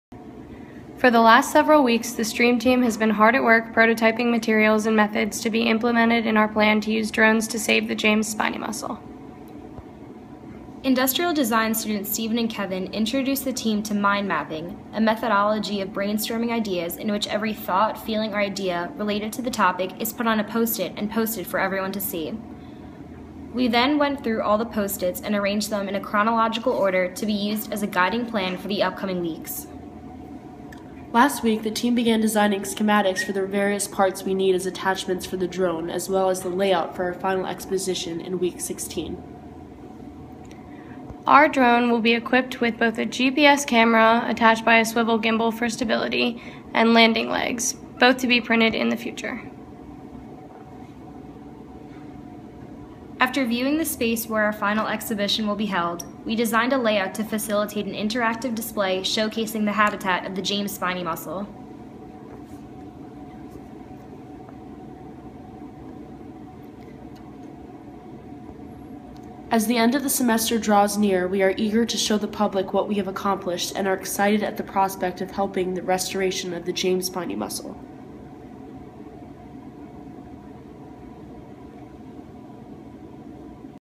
Stream Team Update
Take a look at what the Stream Team has been working on, and their plans for the upcoming showcase event!